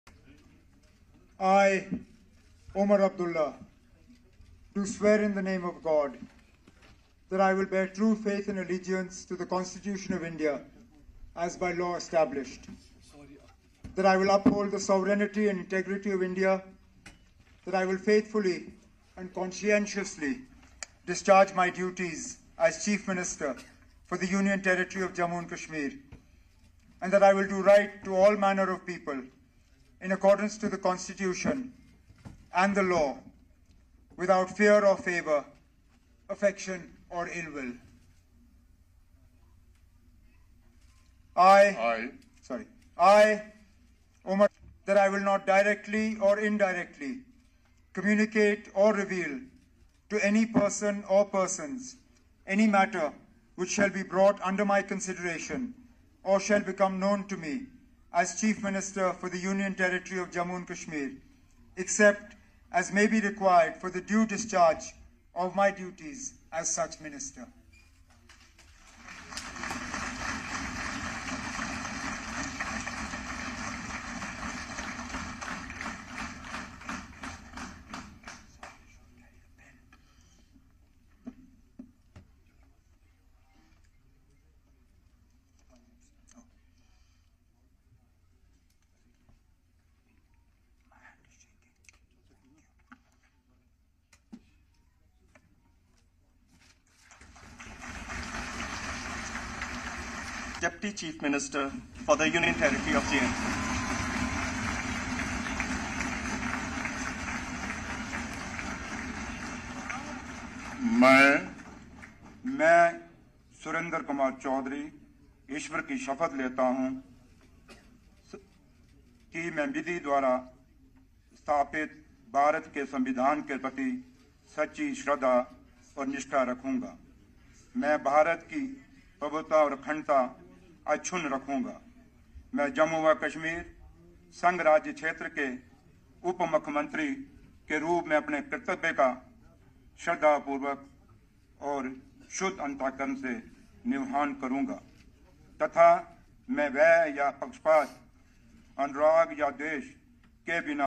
Omar Abdullah Oath Ceremony Live : उमर अब्दुल्ला सरकार में कौन-कौन बना वज़ीर देखिए ? | Breaking News
उमर अब्दुल्ला ( Omar Abdullah ) ने जम्मू-कश्मीर के मुख्यमंत्री ( Jammu Kashmir CM ) के रूप में शपथ ली। लोकसभा ( Lok Sabha ) में विपक्ष के नेता राहुल गांधी ( Rahul Gandhi ), कांग्रेस नेता प्रियंका गांधी वाड्रा, JKNC प्रमुख फारूक अब्दुल्ला( Farooq Abdullah ), समाजवादी पार्टी प्रमुख अखिलेश यादव ( Akhilesh Yadav ), PDP प्रमुख महबूबा मुफ्ती ( Mehbooba Mufti ), AAP नेता संजय सिंह ( Sanjay Singh ), CPI नेता डी राजा ( D Raja ) सहित INDIA गठबंधन ( India Alliance ) के अन्य नेता यहां मौजूद रहे।